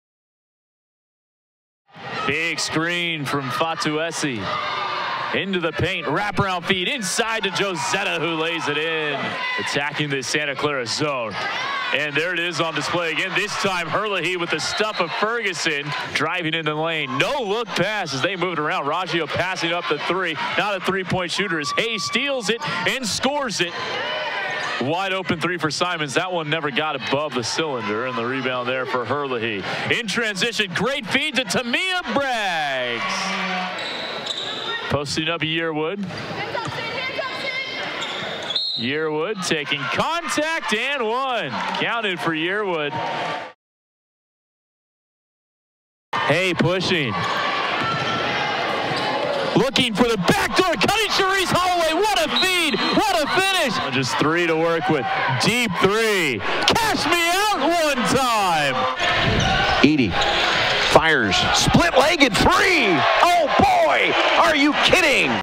Women's Basketball Makes Plays at Both Ends vs. Saint Mary's | Jan. 19, 2019
Catch the best of the Broncos on offense, defense and in transition from the West Coast Conference contest at Leavey Center.